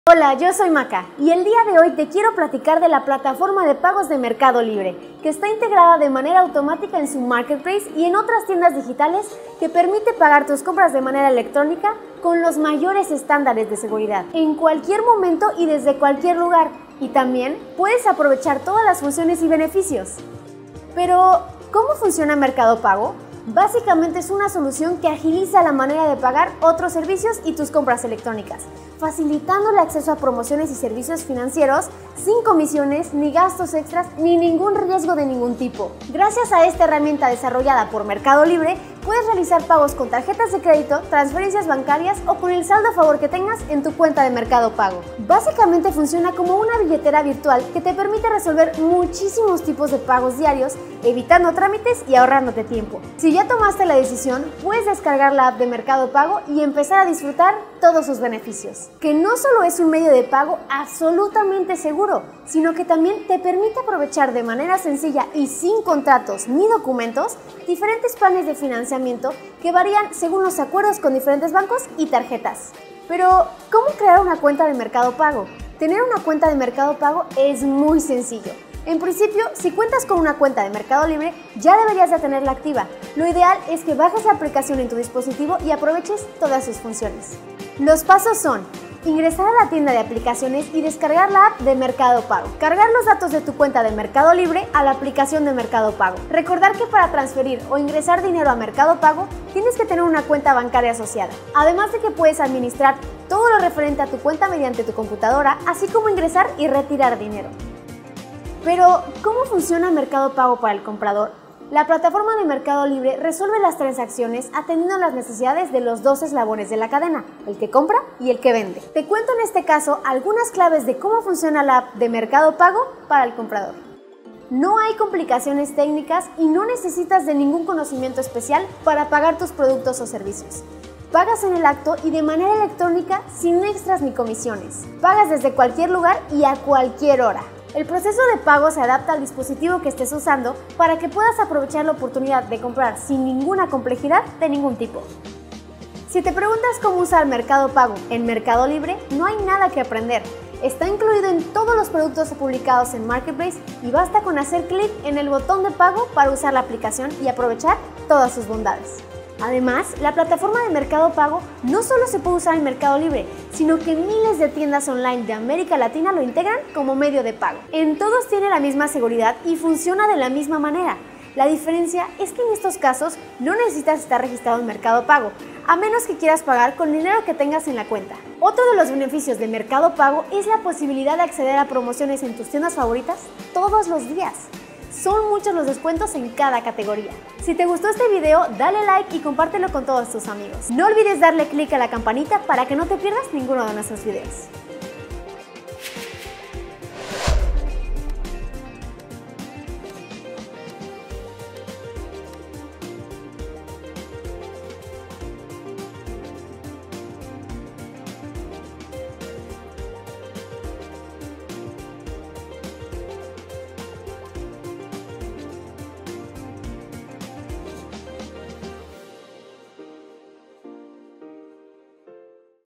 ¿Como funciona Mercado Pago?
Te quiero platicar de la plataforma de pagos de  mercado libre, que está integrada de manera automática en su marketplace y a otras tiendas digitales que permite pagar tus compras de manera electrónica con los mayores estándares de seguridad, en cualquier momento y desde cualquier lugar y también como puedas aprovechar todas sus funciones y beneficios.

Es una solución que te agiliza la manera de pagar otros servicios y tus compras electrónicas, facilitando el acceso a promociones y servicios financieros sin comisiones, ni gastos extras, ni riesgos de ningún tipo.
Gracias a esta herramienta, desarrollada por Mercado Libre, puedes realizar  pagos con tarjetas de crédito, con transferencias bancarias o con saldo a favor que tengas en tu cuenta de Mercado Pago.